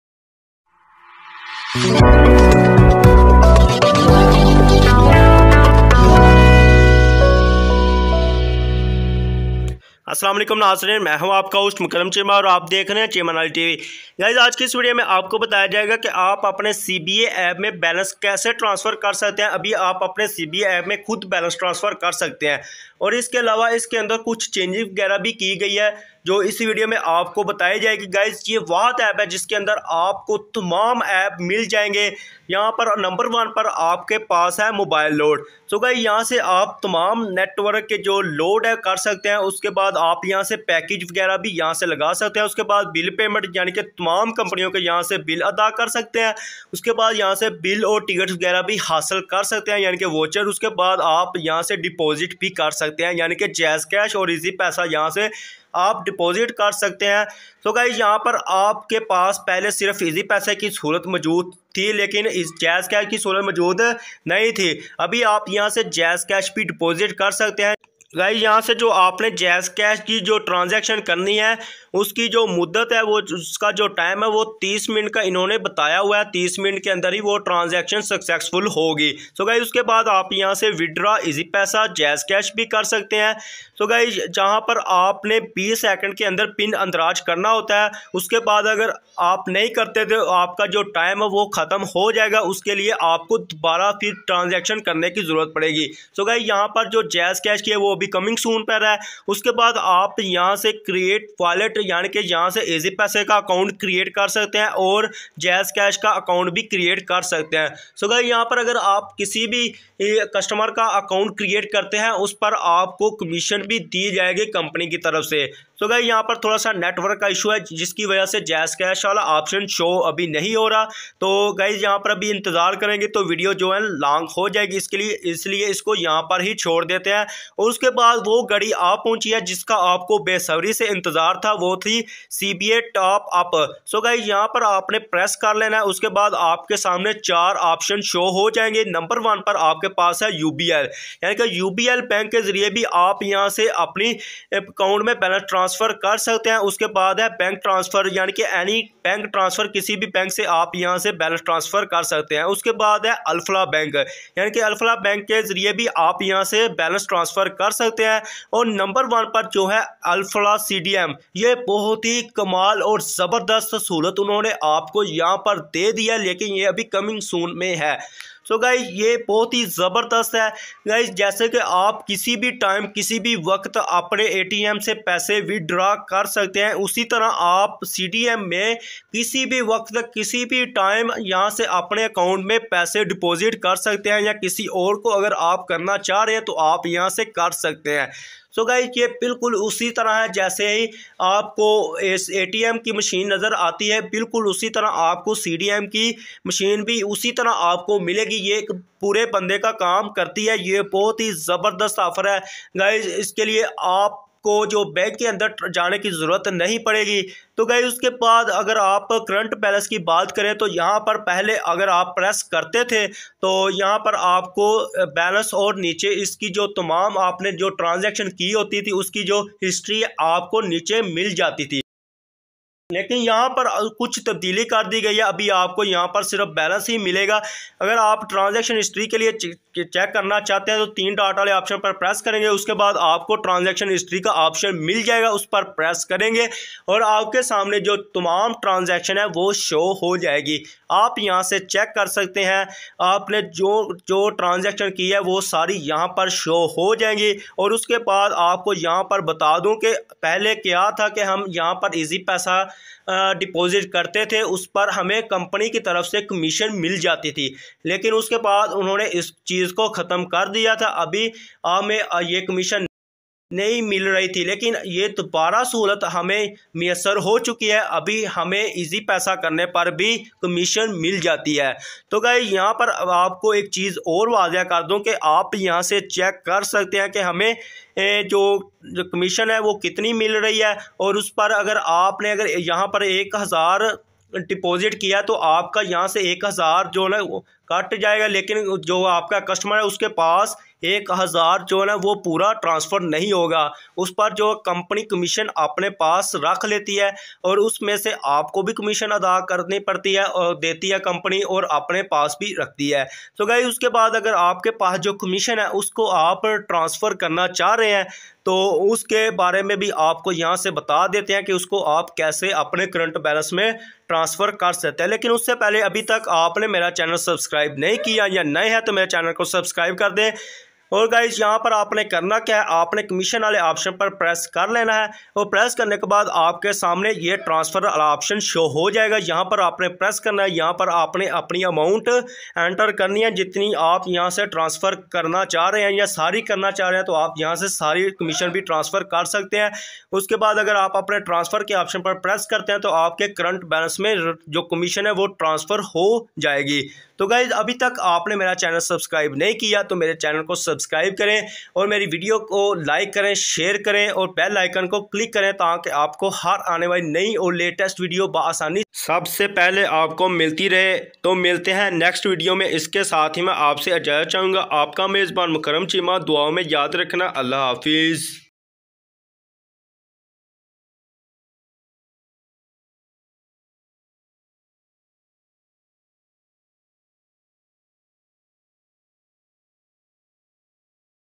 CBA balance khud transfer Kare | CBA App se jazzcash bhi Kare | tamam network load aur package Kare
Cheema knowledge TV

CBA App se tamam network ke load Karen
CBA App se tamam network a packages bhi
kare
CBA app sa tamam utility bill pay kar sakte hai
CBA App se aap easypaisa aur jazzcash Bi deposit kar sakte hain
CBA App se of easypaisa withdrawal bhi kar sakte hain
CBA App se Bulk payment /Bil bhi Ada kar sakte hain
CBA App se AP create wallet bhi kar sakte hain
CBA App se ab aap khud balance transfer kar sakte hain